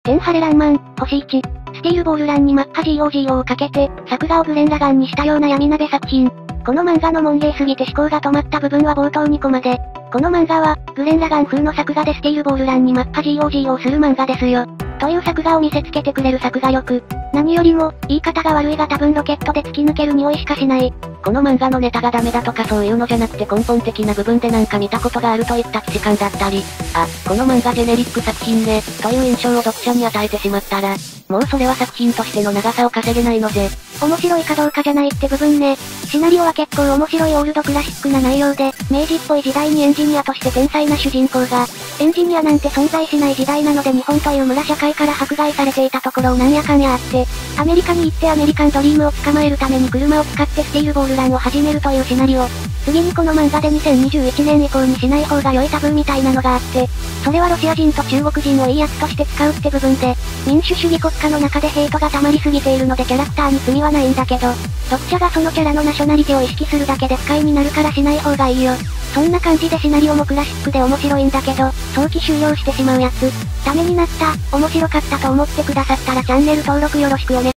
おすすめ漫画!! 天晴爛漫！ あらすじ / アントンシク APPERRACING
天晴爛漫！、☆１

スティールボールランにマッハGOGOをかけて、作画をグレンラガンにしたような闇鍋作品

この漫画のモンゲーすぎて思考がとまった部分は冒頭２コマで、この漫画は、グレンラガン風の作画でスティールボールランにマッハGOGOする漫画ですよ！という作画を見せつけてくれる作画力

何よりも、言い方が悪いが多分ロケットで突き抜ける匂いしかしない

この漫画のネタがダメだとかそういうのじゃなくて根本的な部分でなんか見た事があるといった既視感だったり、「あ、この漫画ジェネリック作品ね」、という印象を読者に与えてしまったらもうそれは作品としての長さを稼げないのぜ

面白いかどうかじゃないって部分ね

シナリオは結構面白いオールドクラシックな内容で、明治っぽい時代にエンジニアとして天才な主人公が、エンジニアなんて存在しない時代なので日本という村社会から迫害されていたところをなんやかんやあってアメリカにいってアメリカンドリームを捕まえる為に車を使ってスティールボールランを始めるというシナリオ

次にこの漫画で２０２１年以降にしないほうが良いタブーみたいなのがあって、それはロシア人と中国人をいい奴として使うって部分で、民主主義国家の中でヘイトが溜まりすぎているのでキャラクターに罪は無いんだけど、読者がそのキャラのナショナリティを意識するだけで不快になるからしない方がいいよ

そんな感じでシナリオもクラシックで面白いんだけど、早期終了してしまう奴